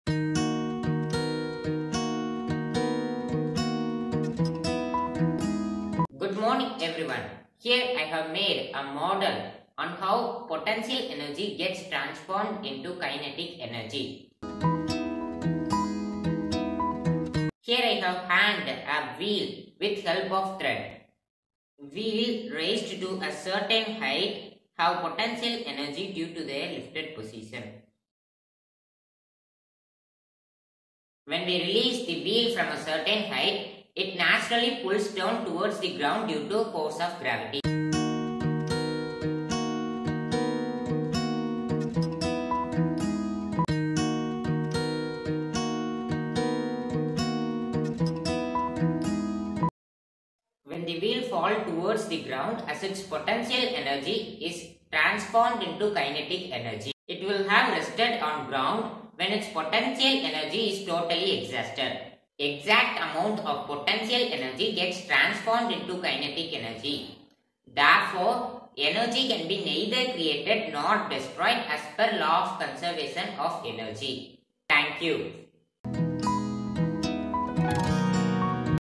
Transformation of Potential Energy into Kinetic Energy ǁ Physics Activity ǁ Hemjo Creations
In this video, the experiment shows transformation of potential energy into kinetic energy is shown
Hemjo Creations